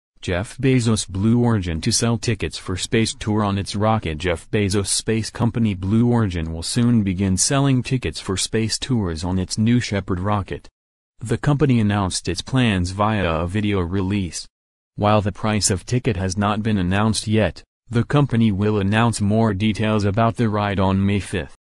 Jeff Bezos' Blue Origin to sell ticketsfor space tour on its rocket
for space tour on its rocket
Jeff Bezos' space company Blue Origin will
soon begin selling tickets for space tours
on its New Shepard rocket. The company
announced its plans via a video release.
While the price of ticket has not been
announced yet, the company will announce
more details about the ride on May5.